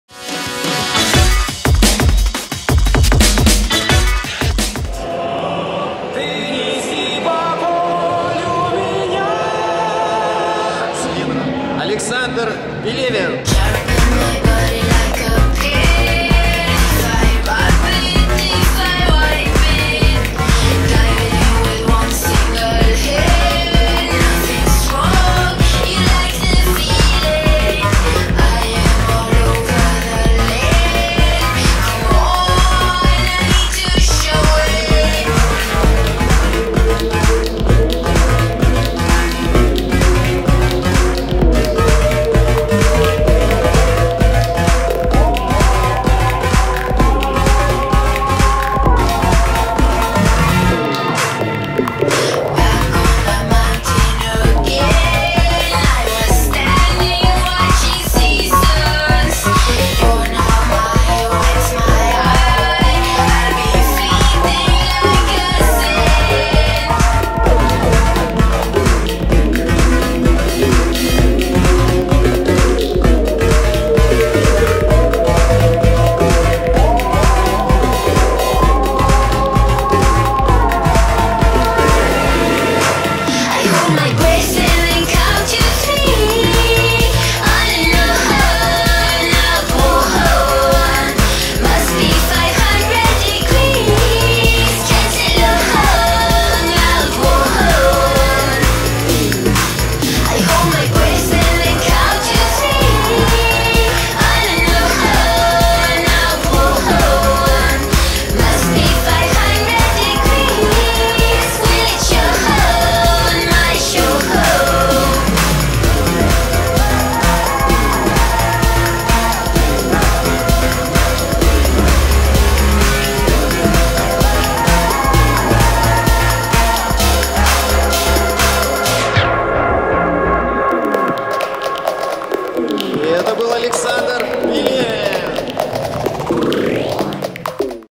RYYC 2010 Afterparty Contest - 1A - 1st - Alexander Pelevin
18 декабря 2010 года состоялся "RYYC 2010 AFTERPARTY CONTEST", организованный компанией Gyroscope.Ru при генеральной поддержке AERO-YO, в Москве, в рамках III Фестиваля Неолимпийских видов спорта.

Дисциплина: Йо-йо Фристайл 1А
1 место
Александр Пелевин